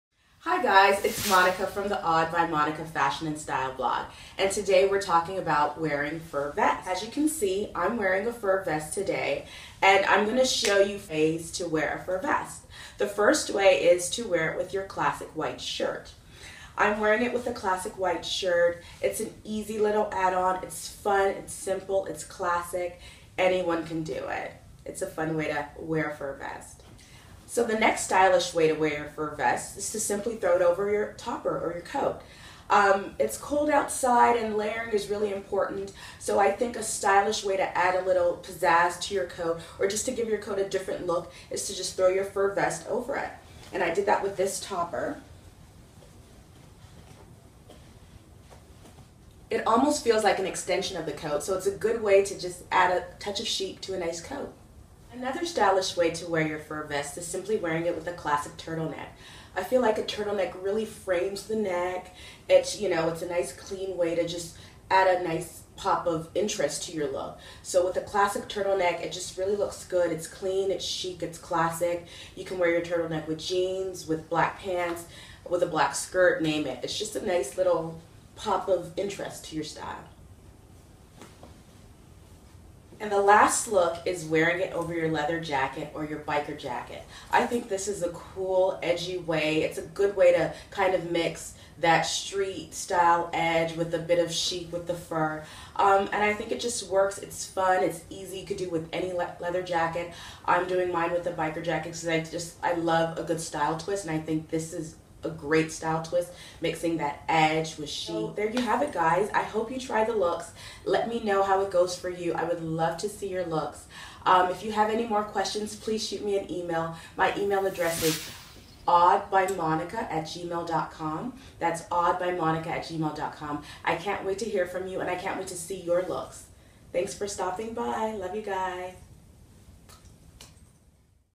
4 STYLISH WAYS TO WEAR A FUR VEST THIS SEASON
Real or faux, just make sure you rock a fur vest this season! Here are 4 stylish and easy ways to stand out in the crowd with your fur vest this season! GO TO THE BLOG TO SHOP MY PICKS FOR RECREATING ALL 4 LOOKS!
FUR VEST
BIKER JACKET
TOP COAT
TURTLE NECK